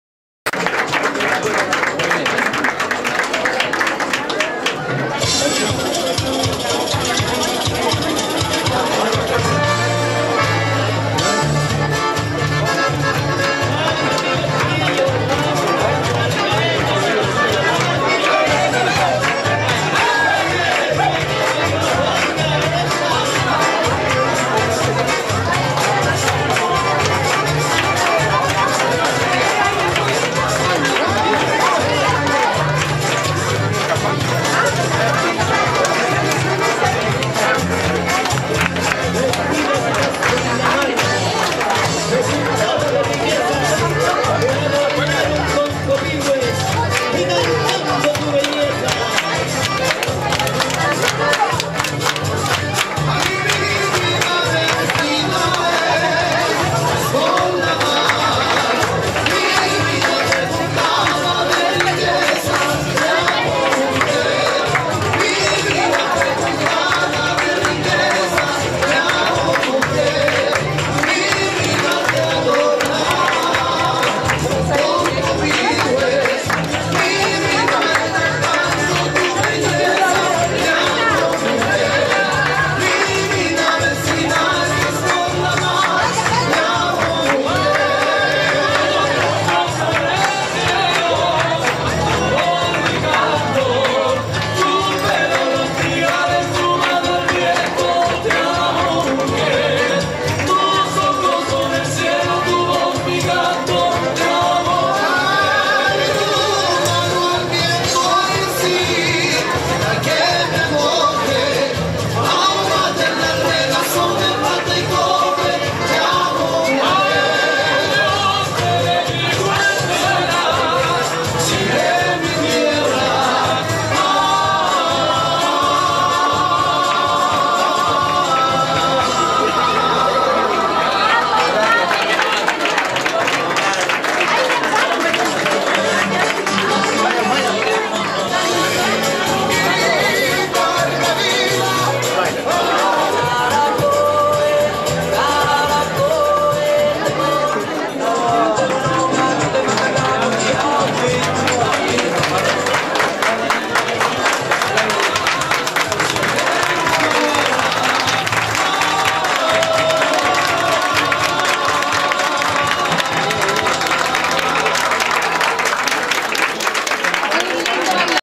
Precentacion de baile en la Embajada de Chile en Japon 4 parte
Final de la Precentacion,baile de cueca con los invitados.